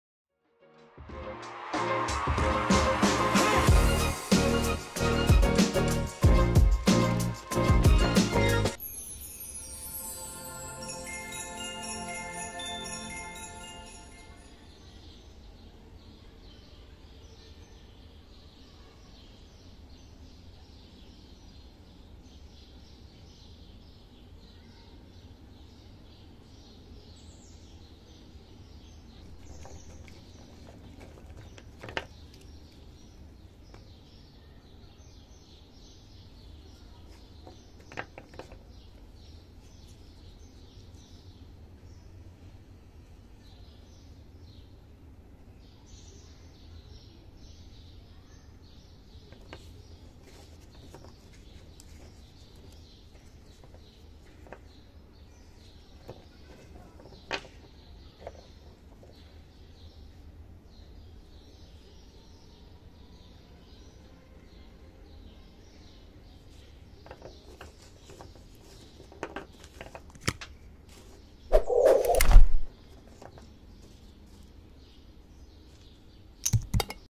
Lehm Ton Kirschsouffle mit Kreide
Lehm Ton Kirschsouffle mit Kreide hat ein erdiges und staubiges Aroma, das an eine frische Erde nach Regen erinnert. Die Struktur von Lehm Ton Kirschsouffle mit Kreide ist knusprig, zerfällt leicht in kleine Kreide-Ton-Krümel. Die Dichte ist leicht und luftig.